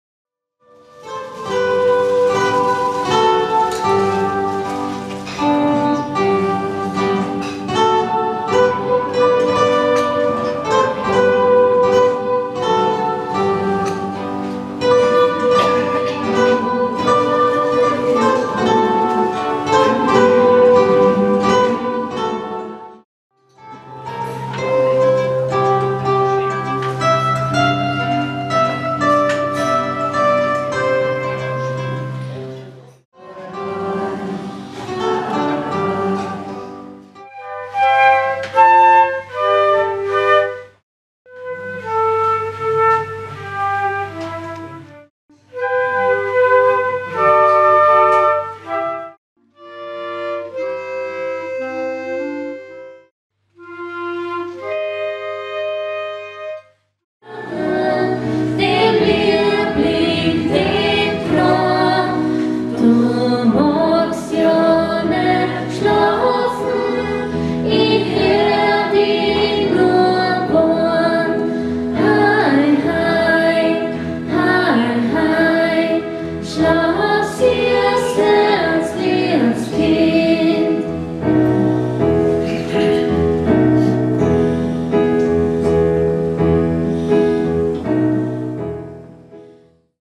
Seniorenweihnachtsfeier der Gemeinde Gutenbrunn am 10.12.2017
Die Gemeinde hatte ihre Senioren zu einer Weihnachtsfeier geladen. Dieses Video zeigt ein paar Szenen von dieser Feier.